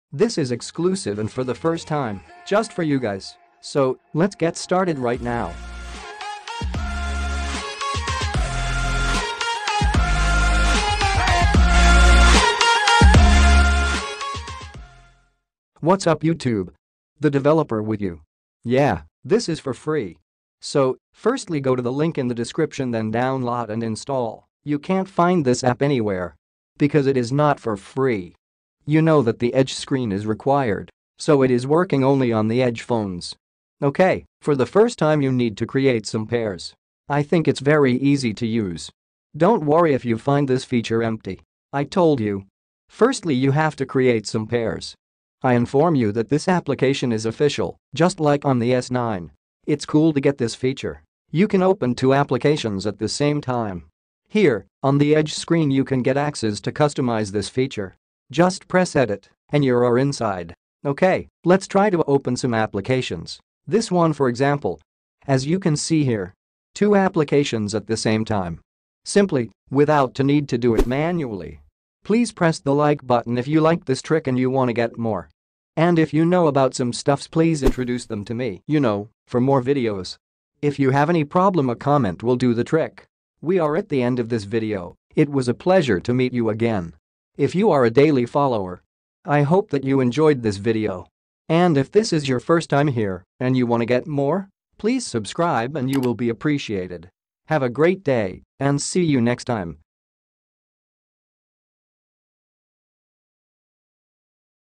Apps Pair Edge Panel for Galaxy S7 edge, S6 edge | S8, Note8 | S9,S9+
This is the Apps Pair for you Edge screen, download, install then enjoy!